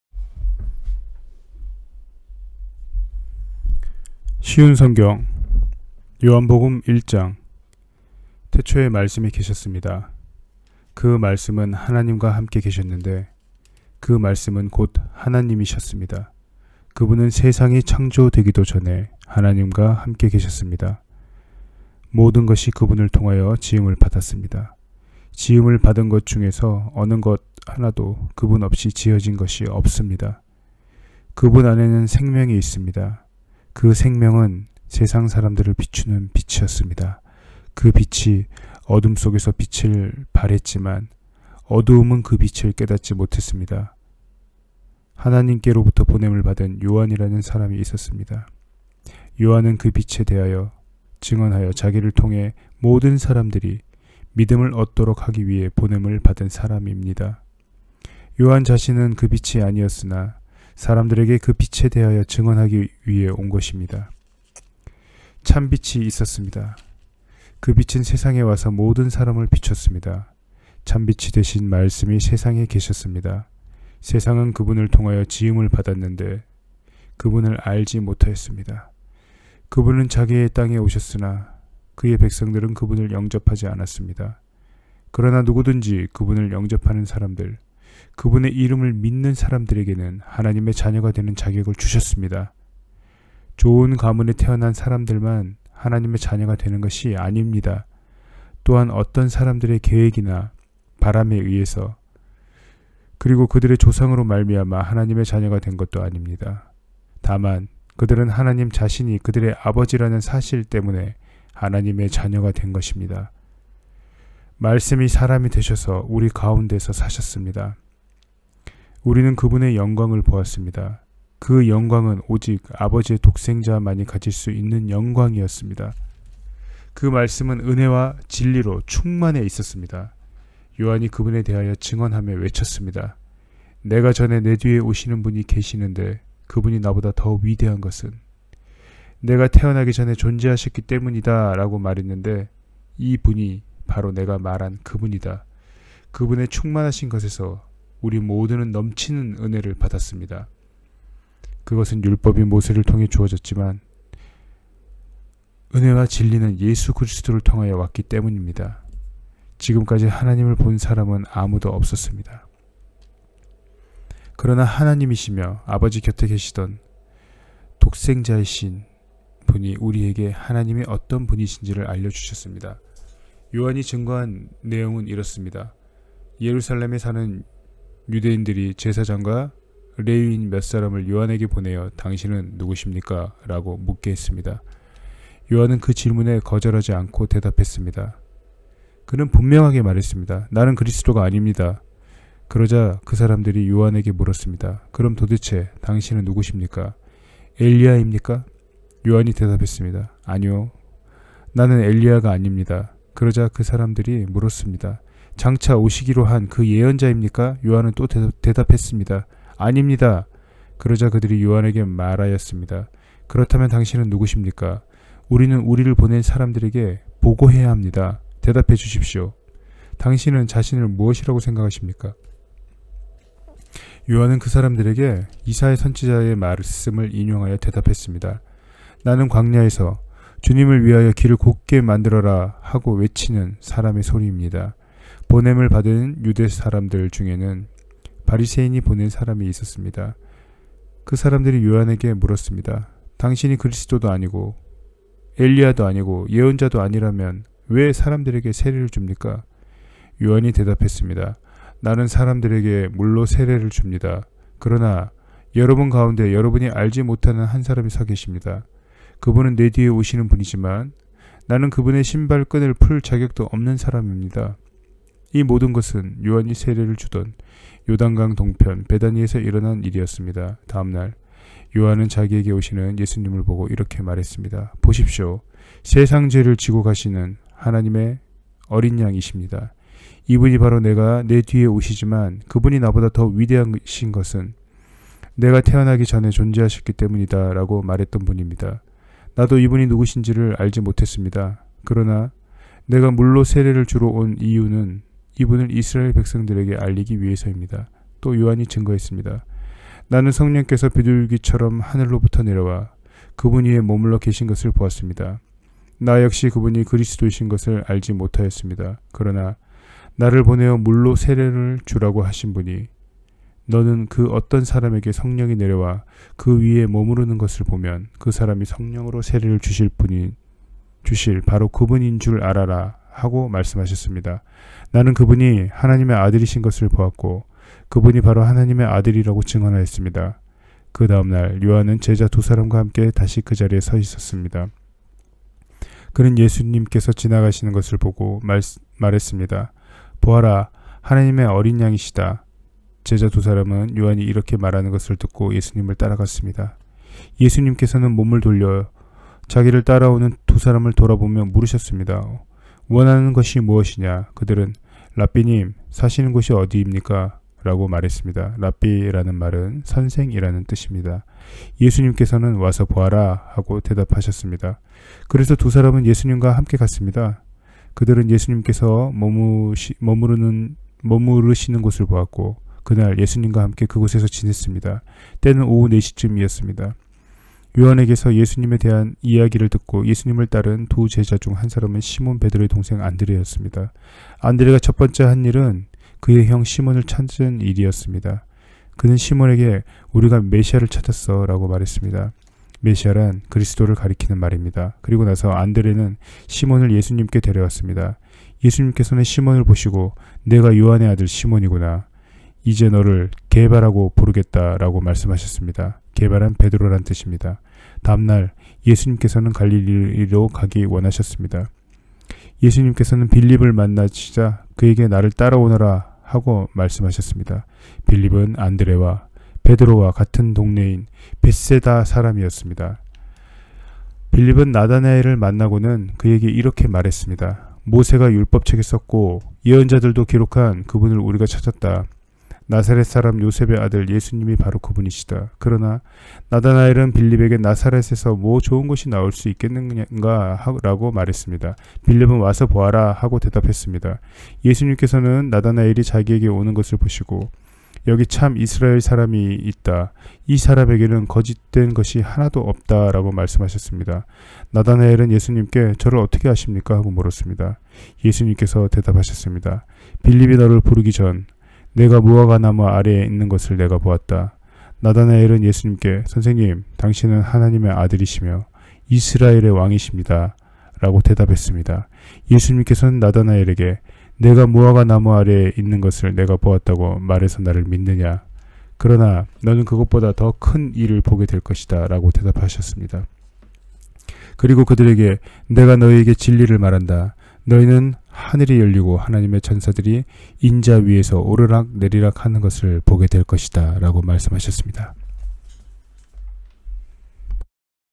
[쉬운성경] 요한복음 1장
바리톤 조배근 성경 통독.
요한복음 1장.